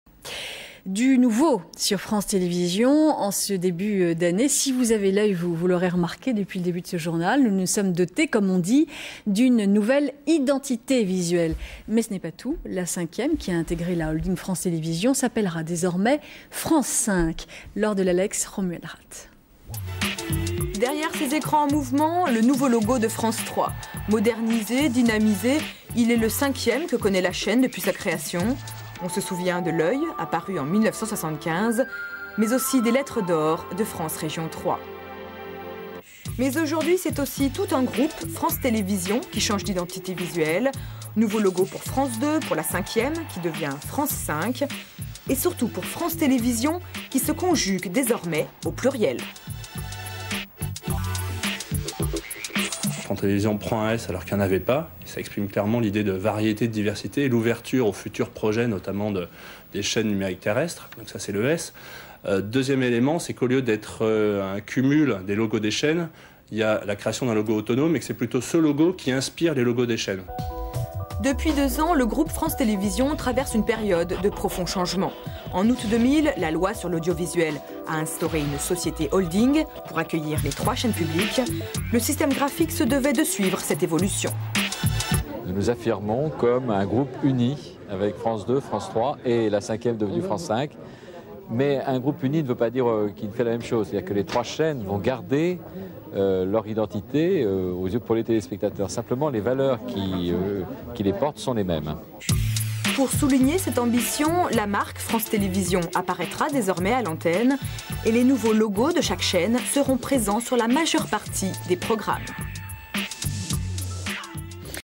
France 2, France 3 et France 5, nouveaux logos 2002 - Archive INA
07 janvier 2002
Le groupe France Télévisions change d'image de marque : France 2, France 3 et la Cinquième devenue France 5 affichent désormais de nouveaux logos. Christophe JARREAU de l'agence de communication Gédéon et Marc TESSIER, président du groupe France Télévisions, expliquent le sens de cette nouvelle identité visuelle.